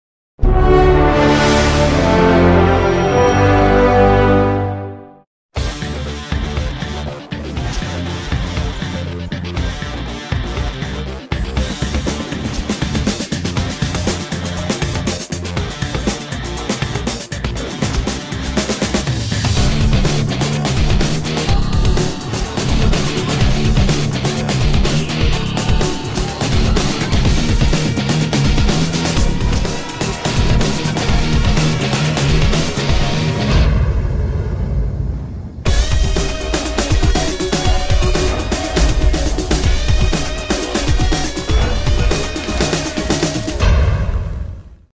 Construction BF109E RC
Bande-annonce des vidéos à venir concernant la construction d'un modèle radiocommandé d'un Messerchmitt BF109E, étape par étape. Mise en lignes prochainement.